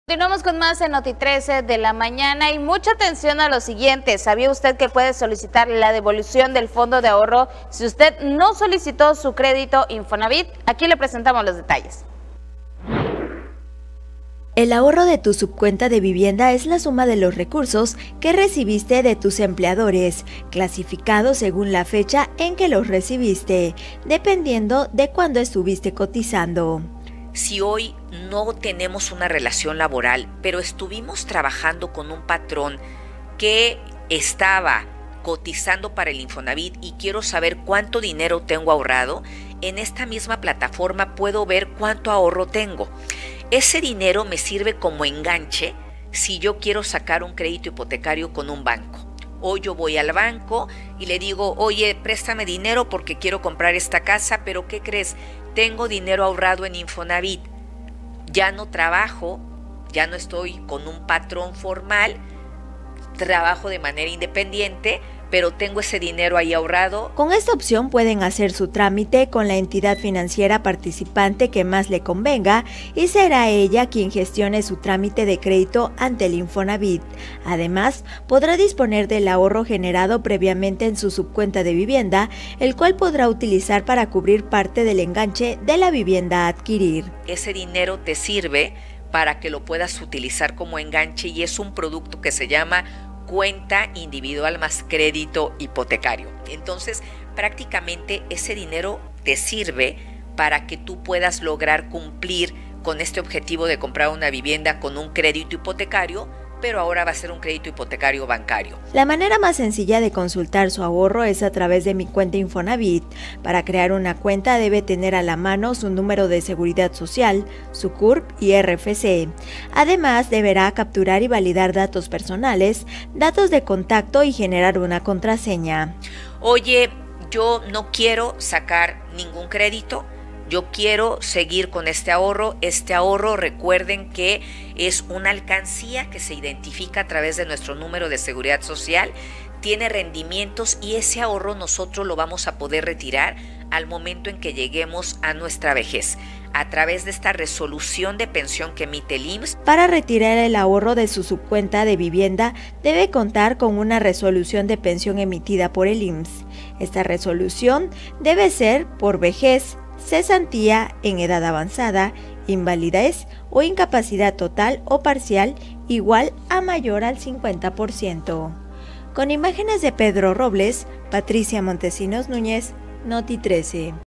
¿Puedes solicitar la devolución del fondo de ahorro, si no utilizaste tu Infonavit? | NOTITRECE
🎥NotaInformativa | Podrás disponer del ahorro generado previamente en su subcuenta de vivienda, el cual podrás utilizar para cubrir parte del enganche de la vivienda a adquirir.

📝Patricia Montesinos